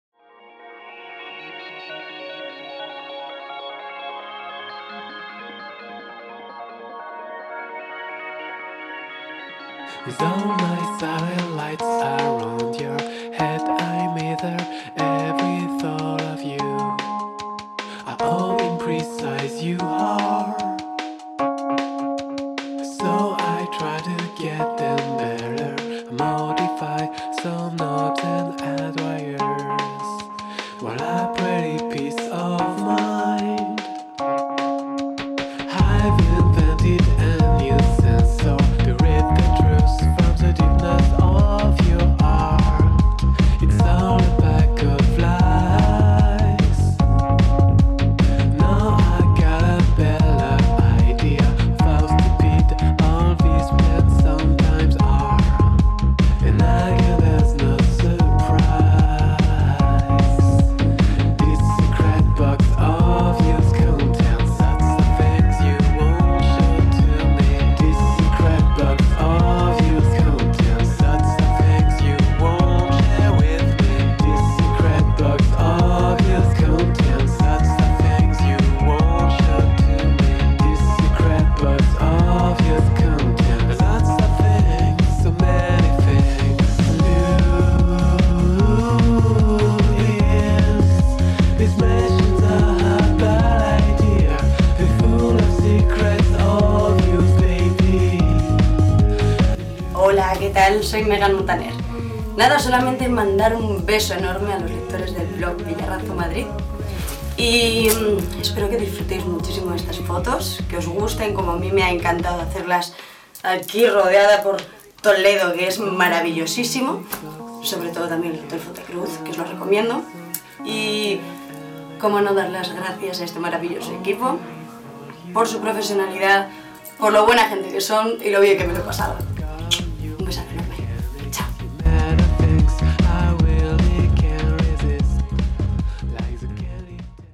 Megan Montaner - Behind the scenes - Revista Semana
Making of realizado por Villarrazo Madrid de la producción de moda que se realizó a la actriz Megan Montaner para la revista SEMANA

Video y montaje: Alejandra Godia
Fotógrafo: Cuco Cuervo
Peluqueria y maquillaje: Paco Orellana para OlHexperience